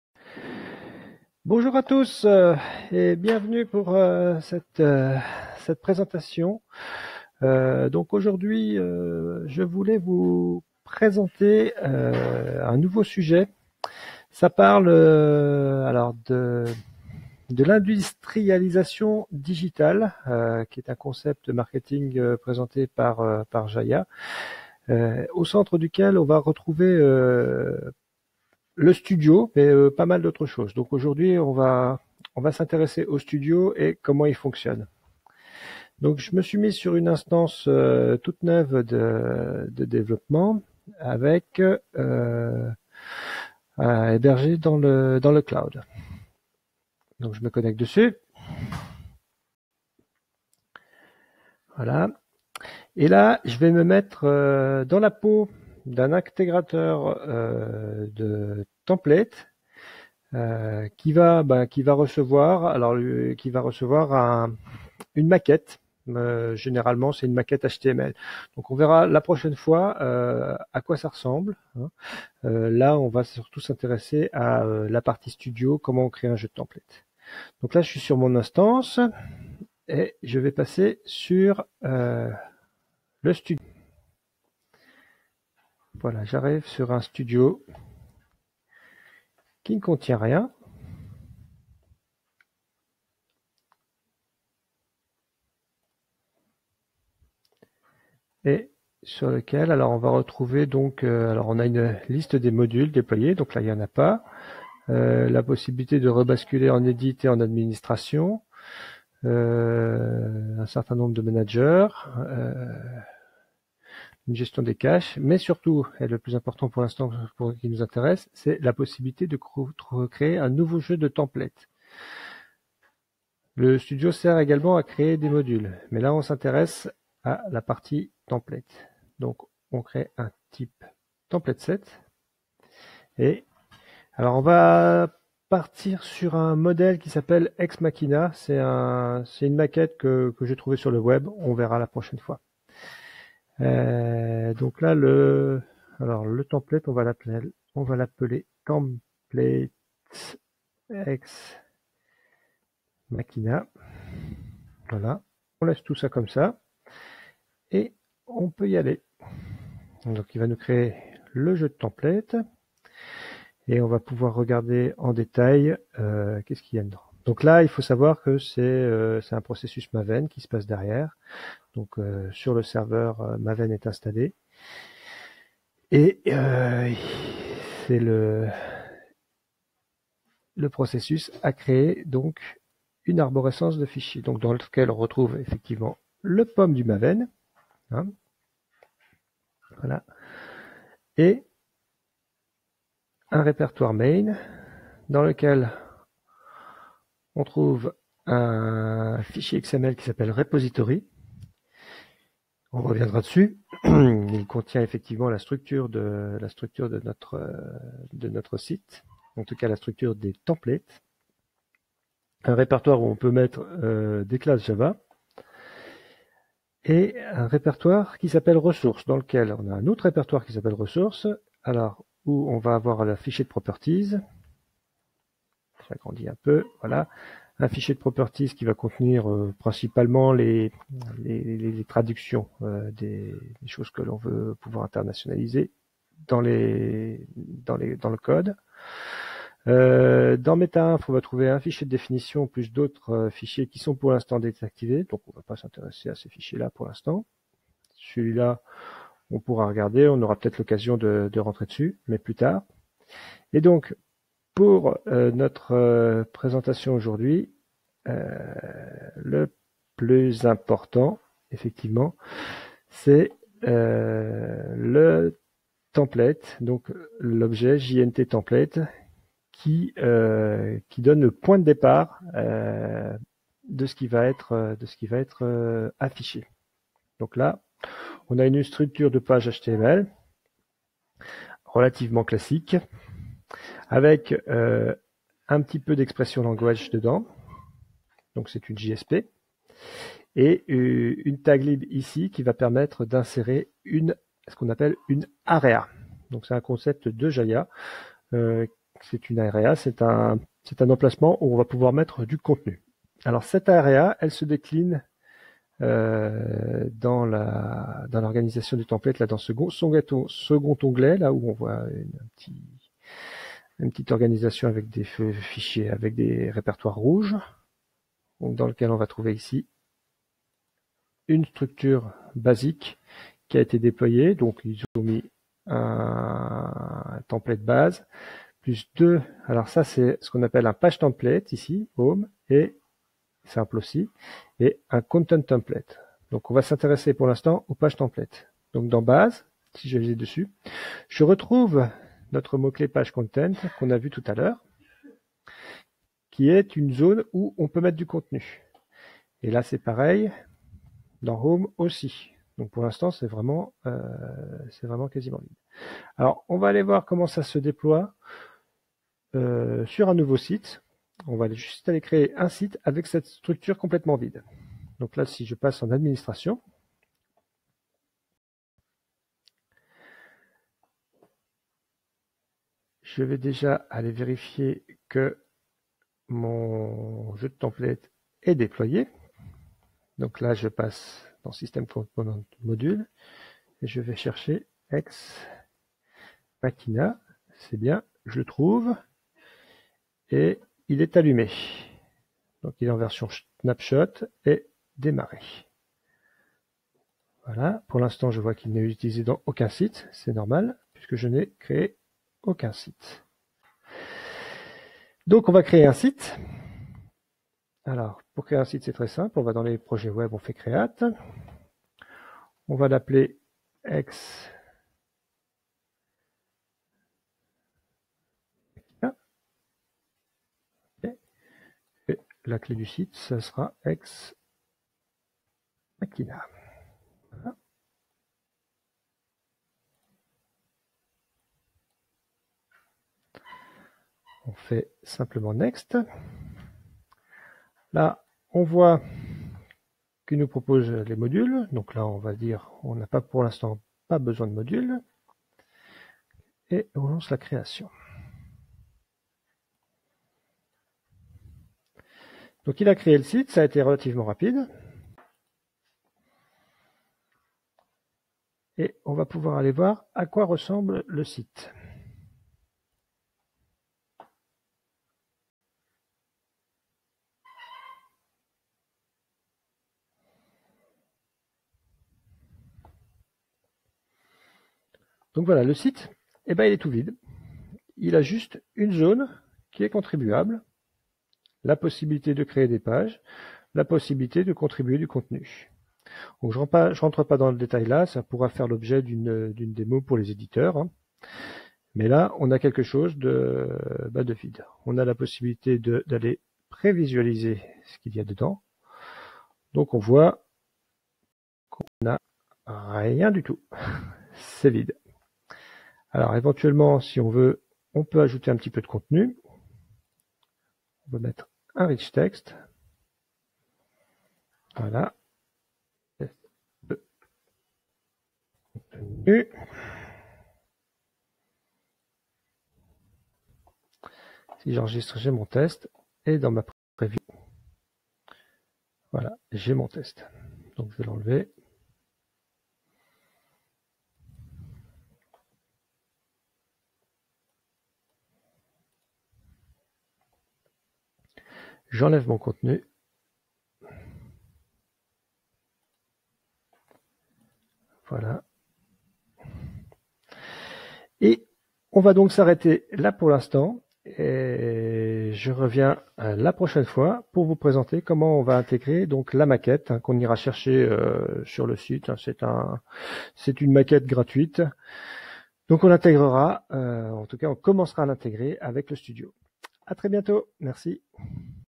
Jahia Community : 1-Premiers pas avec le studio pour les développeurs de templates (Débutant)
Industrialisation digitale avec Jahia.

Dans cette série de vidéos, Christophe Huault démontre la puissance du Studio Jahia en intégrant la maquette HTML "Ex Machina".